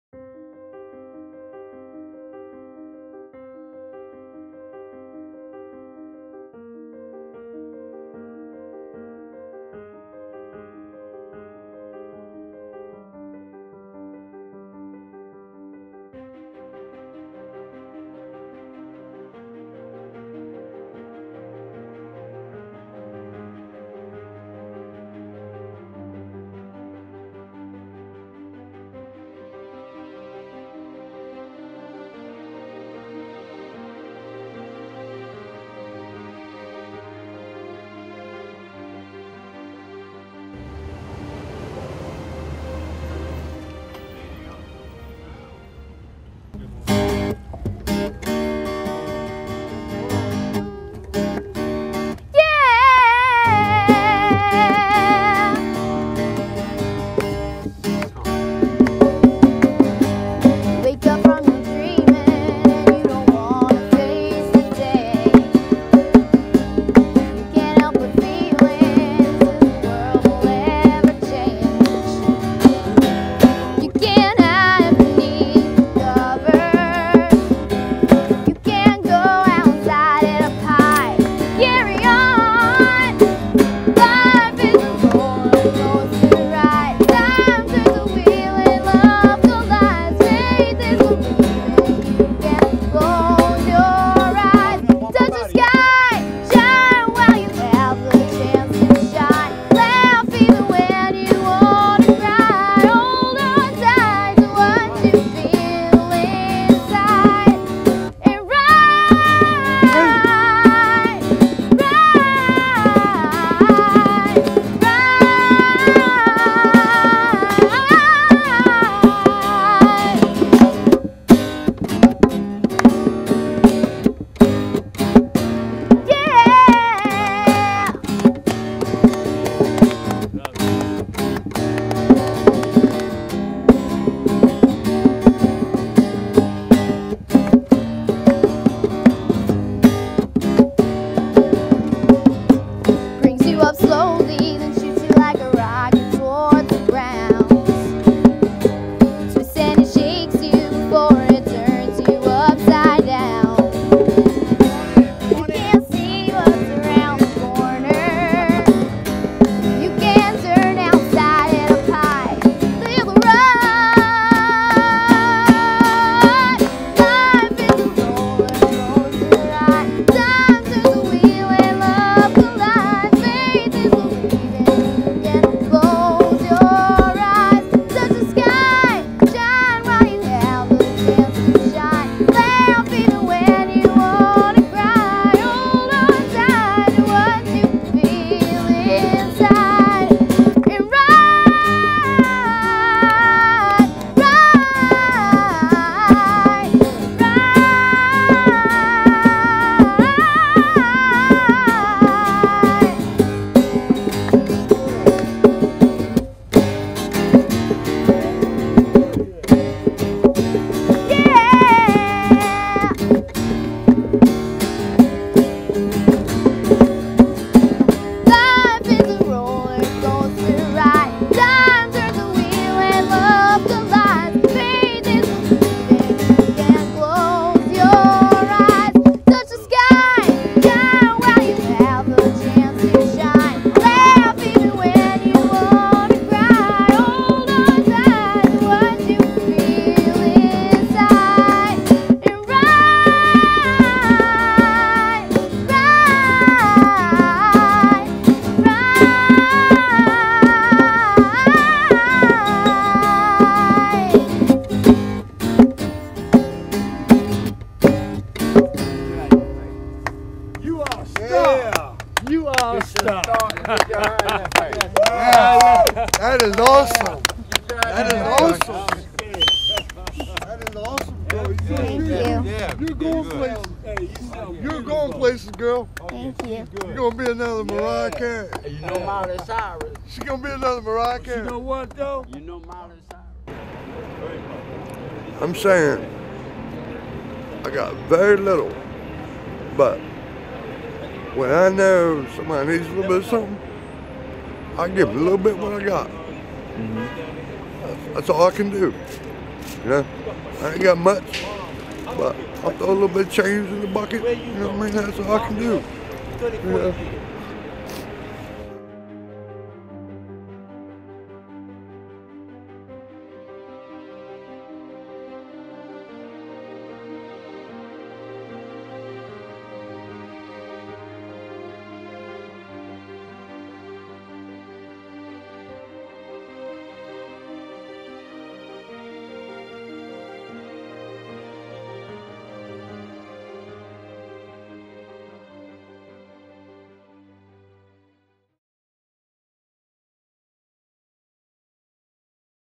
Help Find Rob: We have been trying to thank him since 2009
On Columbus Day 2009, Abby, Madie and Paul went to Union Station in Washington, DC to play music and raise money for Taylor Love who was fighting cancer at the time. When they were ready to go back home to Virginia, a homeless man asked them to play a few songs for him and his friends. A man named Rob did something amazing and we have been trying to thank him for 4 years. The problem is that we can't find him and don't know what happened to him. Can you help?